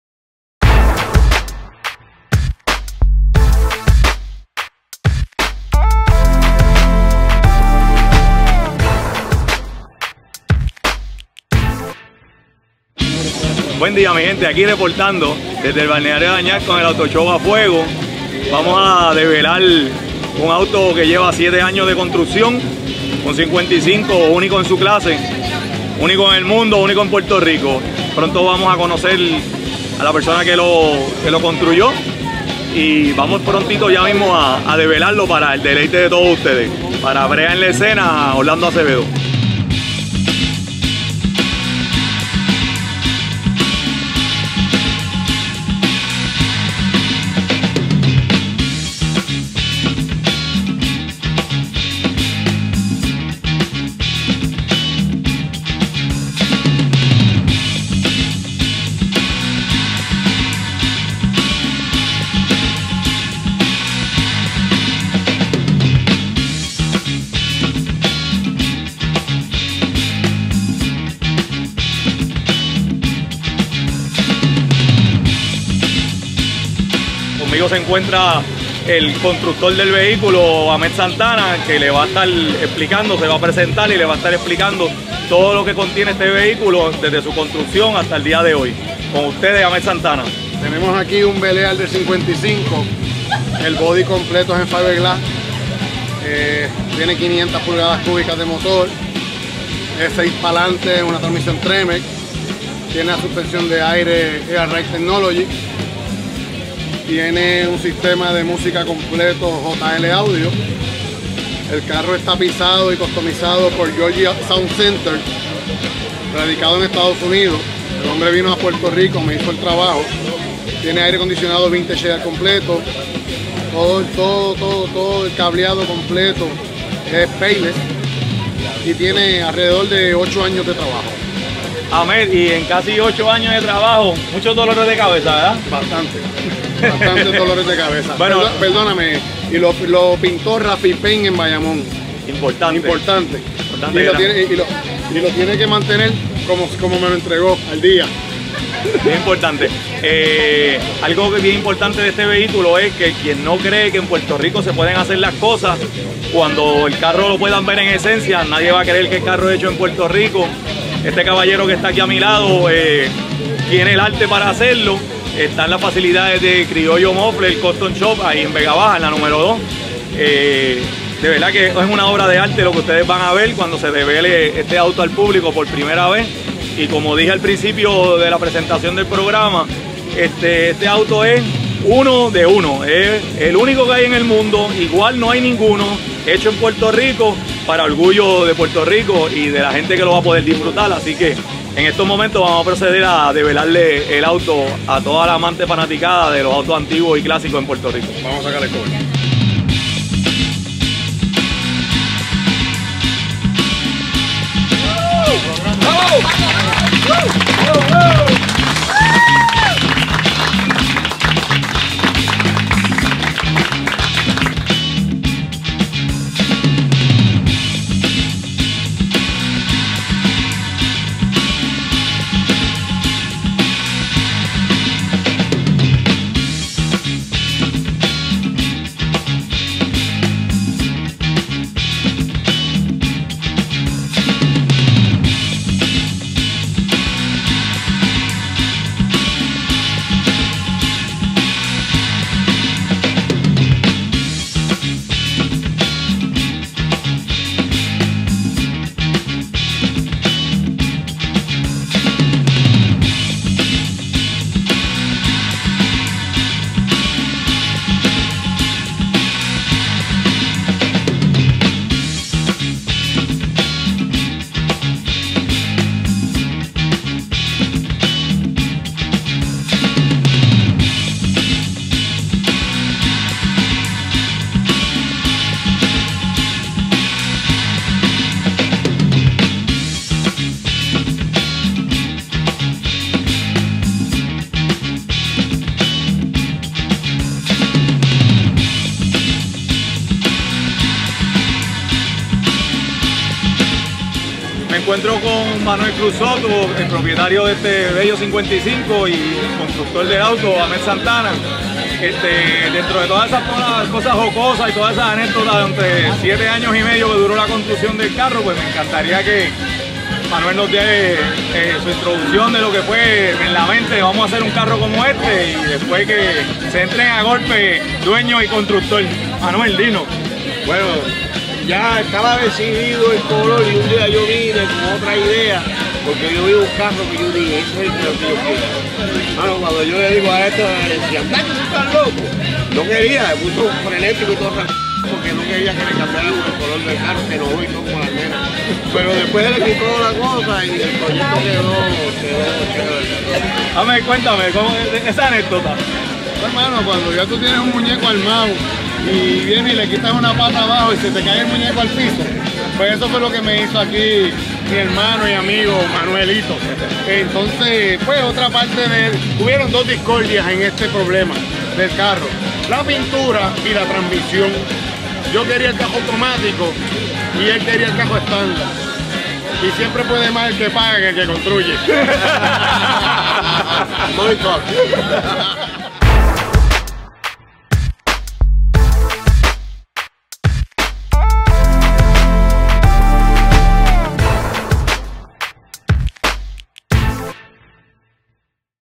BREA "On the Scene" Ep. 8 (2015): COOL/HOT ROD (Autoshow A Fuego)
ENGLISH:
BREA "On The Scene" was present for the unveiling of a very special custom build, 8 years in the making: a 55 all fiberglass chopped top Chevy Bel Air owned by Manuel Cruz Soto.

SPANISH:
BREA "En La Escena" estuvo presente para el develado de un vehículo customizado muy especial, el cual estuvo 8 años en construcción: un Chevy Bel Air del 55 con el techo "choppeado" y la caja en "fiberglass," propiedad de Manuel Cruz Soto.

Presentado Por:
Custom's Autologic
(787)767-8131